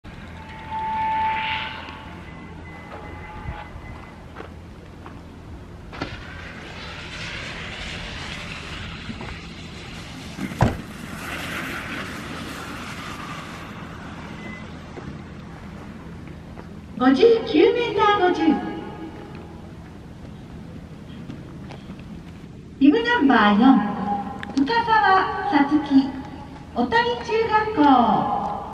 鈴木楓花 選手 第101回全日本ノーマル １本目 59.5メーター
山形三中３年の楓花選手。１本目、女子組31位。
2022/10/21(金) 白馬ジャンプ競技場